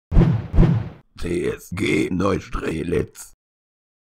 Ich rülpse "TSG Neustrelitz"
Hier seht ihr, wie ich "TSG Neustrelitz" rülpse.